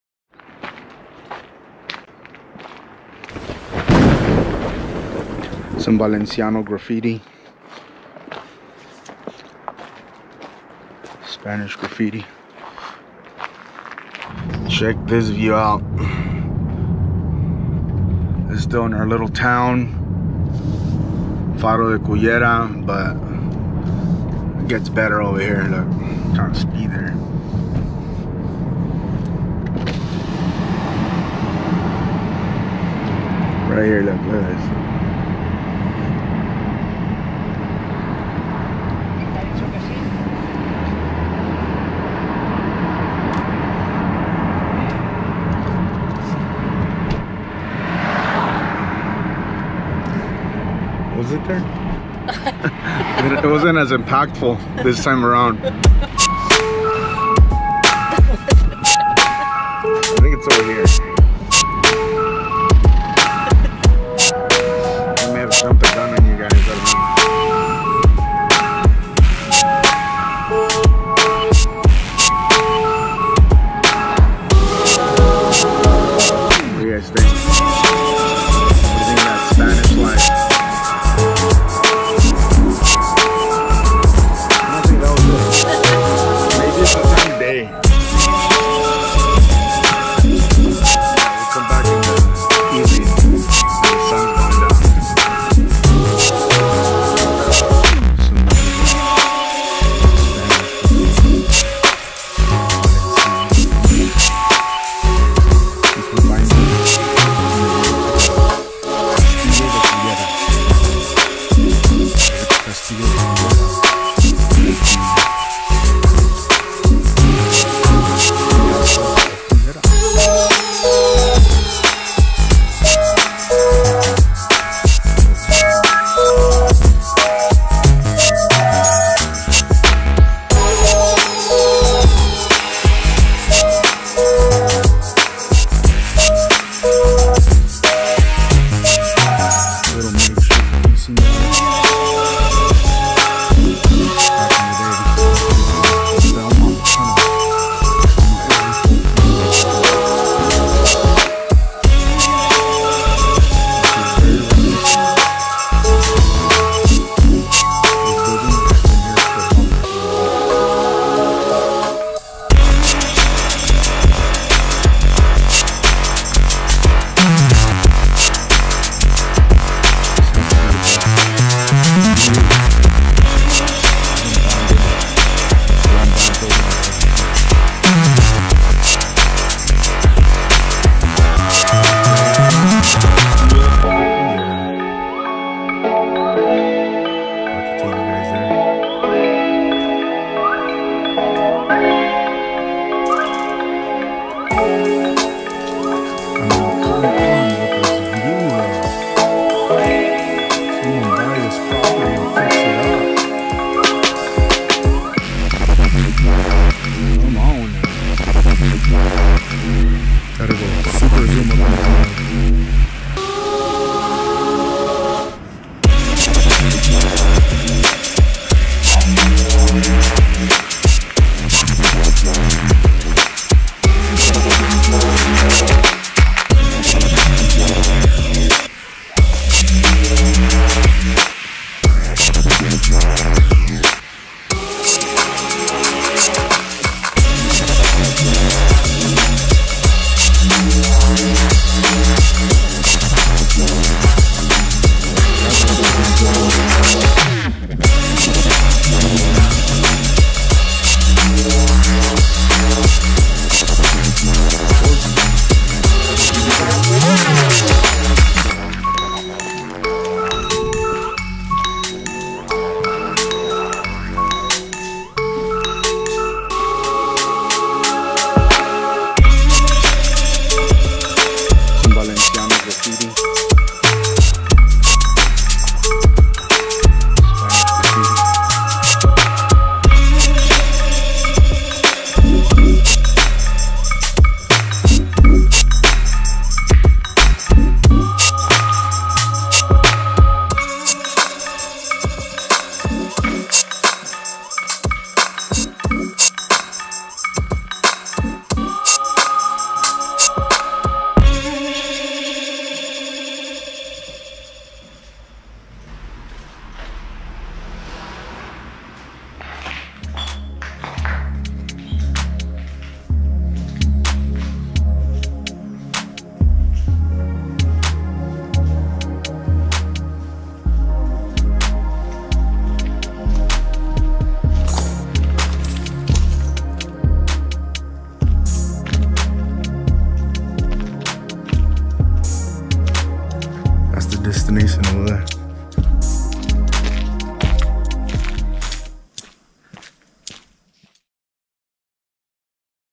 🇪🇸Castillo de Graffito: The Graffiti Yard that Serves As a Retreat | Cullera, Spain
You can find this Graffiti yard on your way to El Castillo de Cullera, with some of the most amazing views you will ever see. Nothing but peace and tranquility up here. It's been here for years.

Pots trobar aquest pati de grafits de camí al Castell de Cullera, amb algunes de les vistes més impressionants que mai veuràs. Res més que pau i tranquil·litat aquí dalt. Ha estat aquí durant anys. Subscriu-te per a més vídeos de grafits d’arreu del món; publicaré un o dos al mes

Puedes encontrar este patio de grafitis de camino al Castillo de Cullera, con algunas de las vistas más asombrosas que verás. Nada más que paz y tranquilidad aquí arriba. Ha estado aquí durante años.